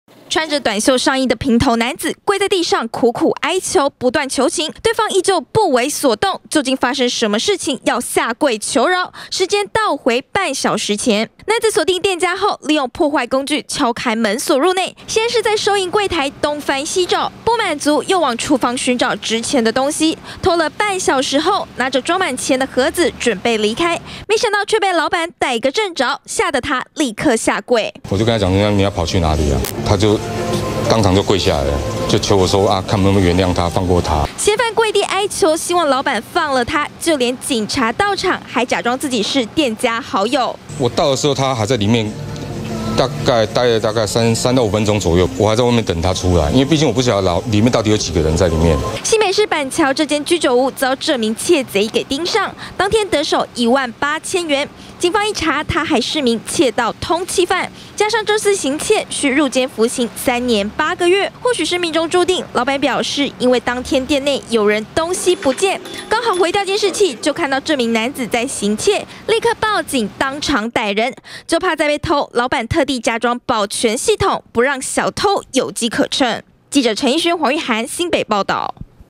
20200316中天新聞 偷東西被逮個正著！ 竊賊嚇到「下跪求饒」
新北市板橋一名竊賊，盯上文化路上一間居酒屋，趁著店家打烊時，利用破壞工具，撬開門鎖入內行竊，得手一萬八千元，正當離開時，卻被店家老闆逮個正著，竊賊嚇到當場下跪求饒。